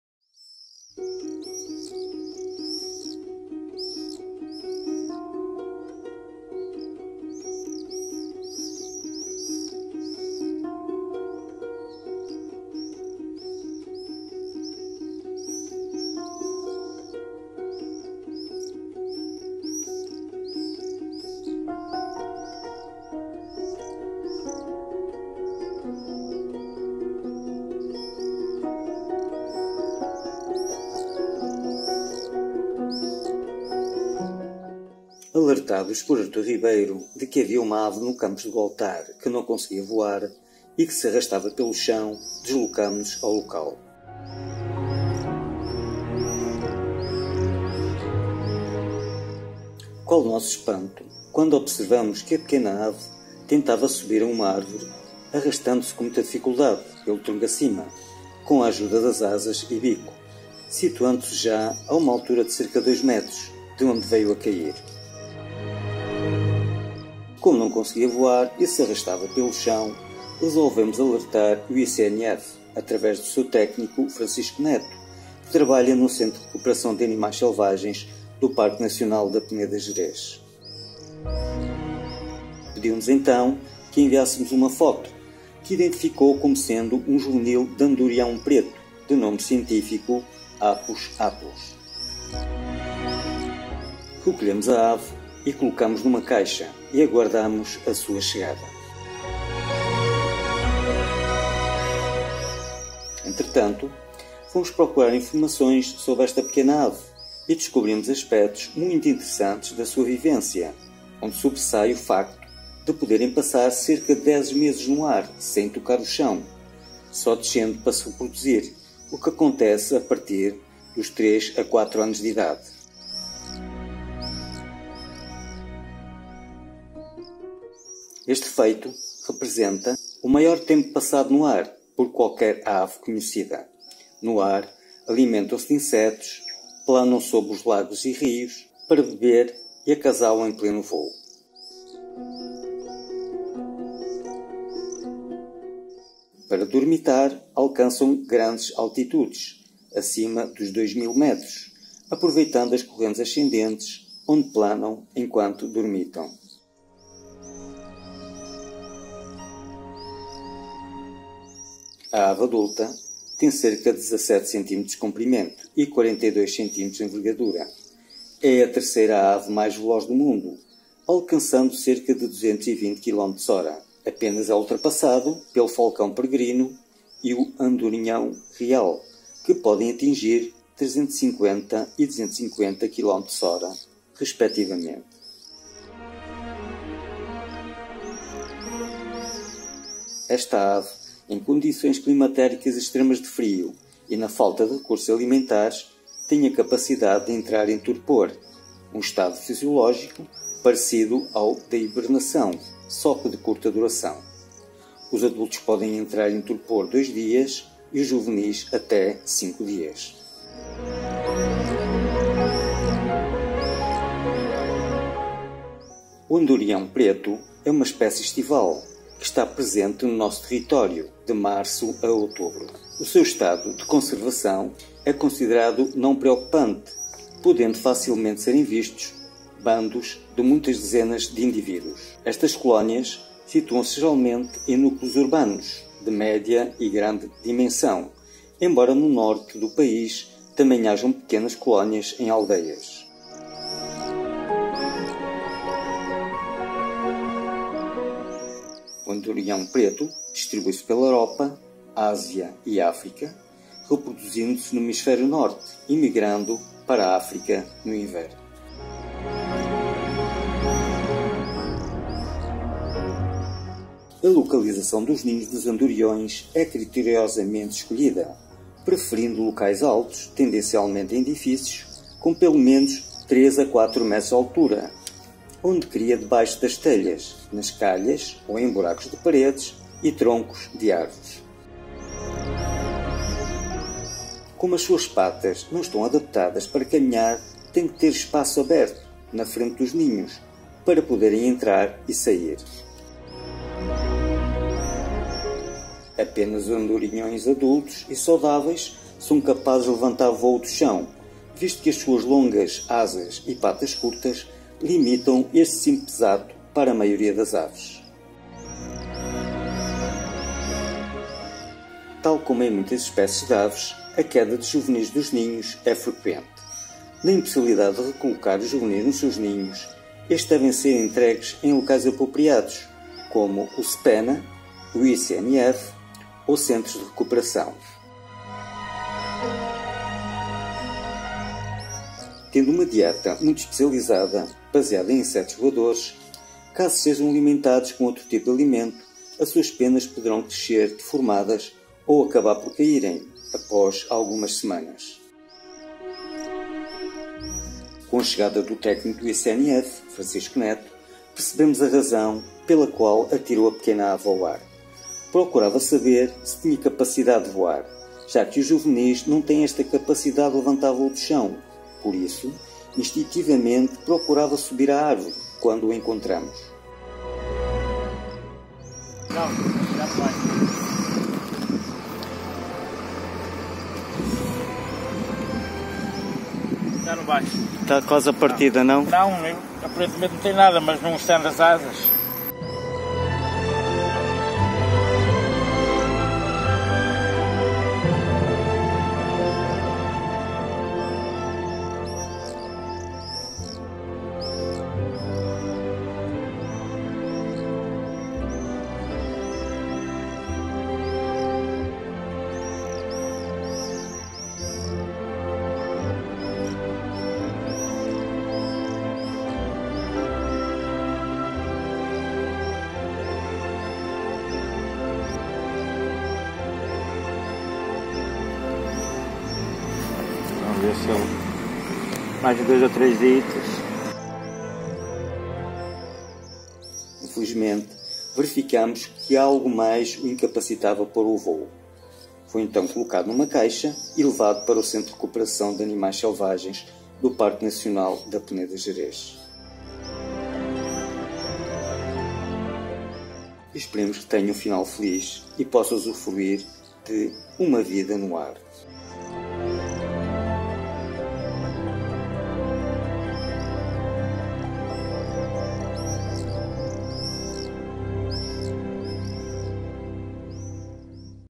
A incrível odisseia de vida do Andorinhão Preto (Apus apus apus)
A incrível odisseia de vida desta ave que visita o nosso país para procriar.  Dez meses anuais são passados sempre a voar. Trata-se de uma subespécie de Apus apus, daí  o seu nome científico Apus apus apus